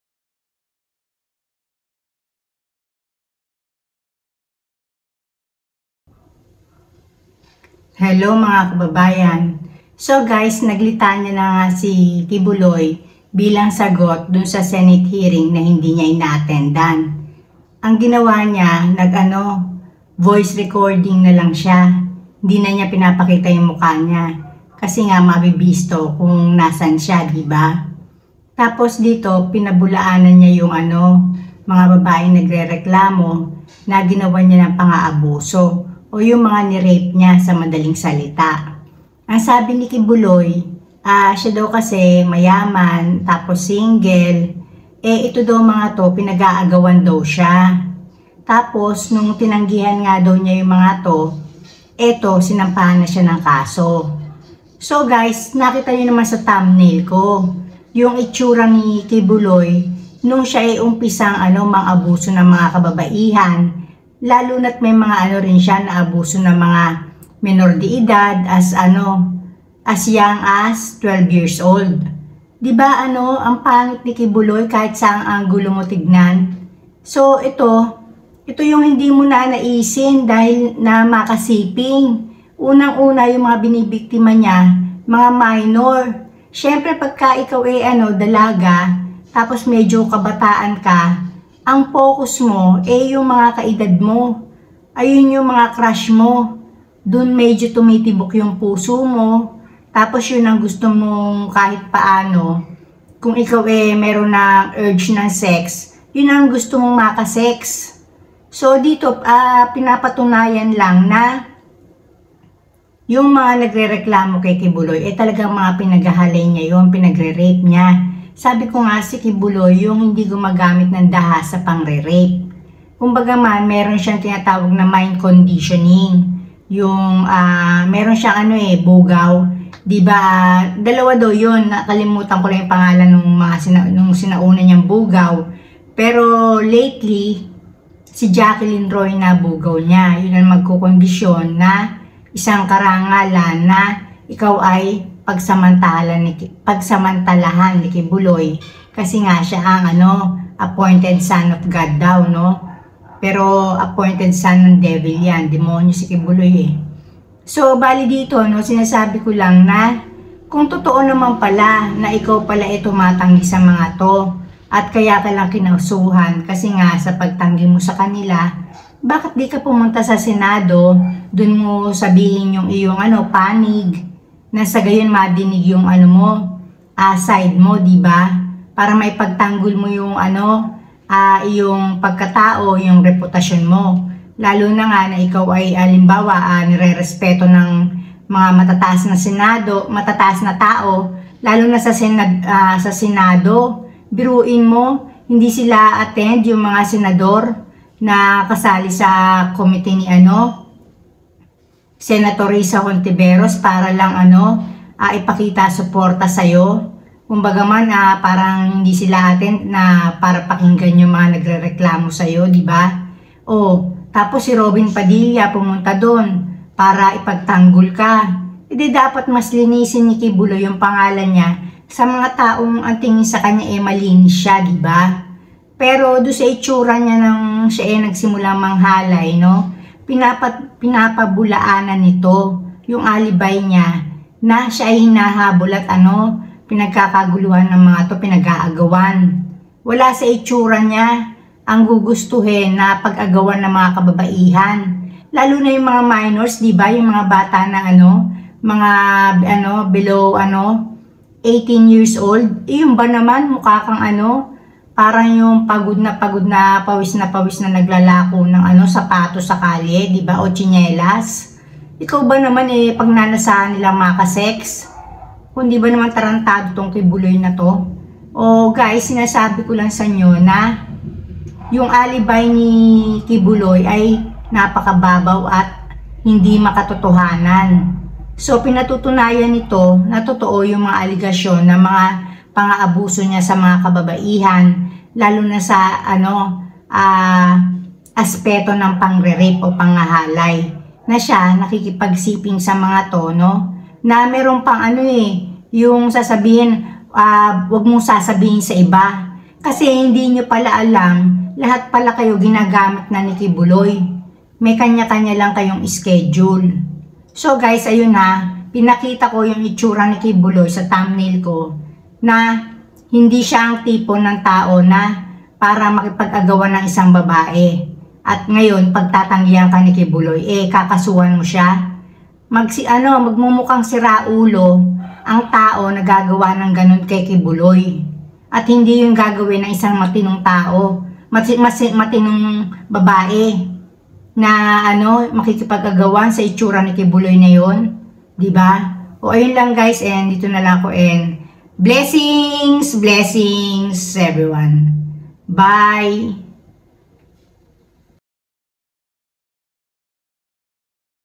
HINDI TOTOO NA PINAG-AAGAWAN NG MGA BABAE SI QUIBOLOY AT TINANGGIHAN NYA MGA TO KAYA SYA KINASUHAN
Hindi totoo ang alibi ni Quiboloy na sya ay pinag-aagawan ng mga babae at tinanggihan nya mga to kaya sya kinasuhan ng mga to.